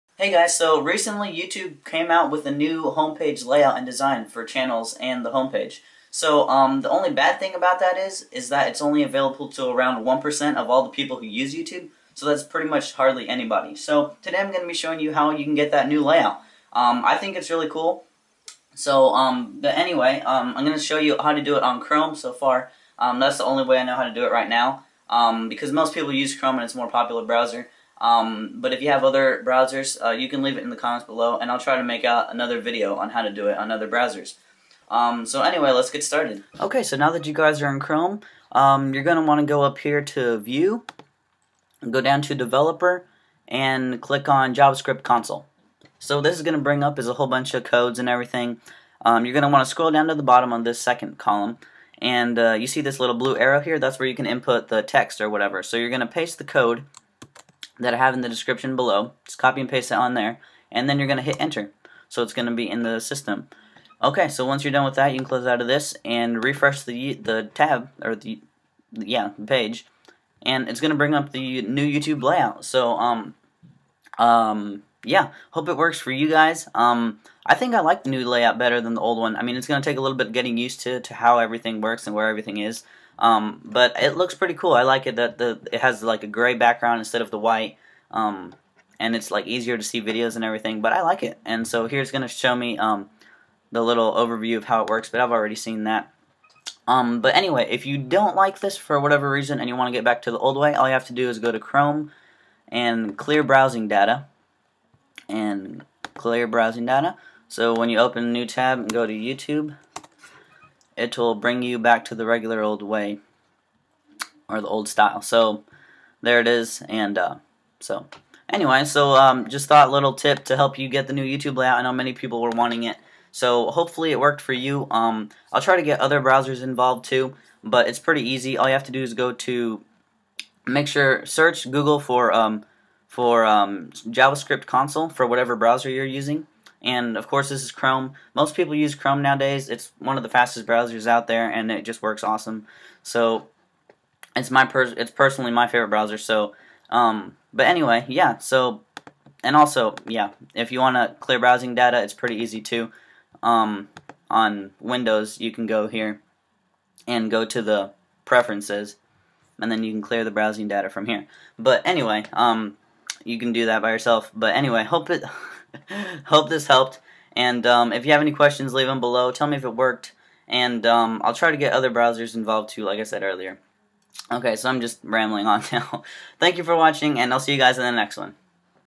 How to get the new YouTube Layout!
Code: document.cookie="VISITOR_INFO1_LIVE=ST1Ti53r4fU"

1.) Open you're browser to YouTube.com

2.) Open the javascript console

3.) Paste this code: document.cookie="VISITOR_INFO1_LIVE=ST1Ti53r4fU"

Then press enter.

4.) Refresh the page and thats it! New Youtube!

How-to YouTube new layout 2011 HD 720p TheJoshlmanTech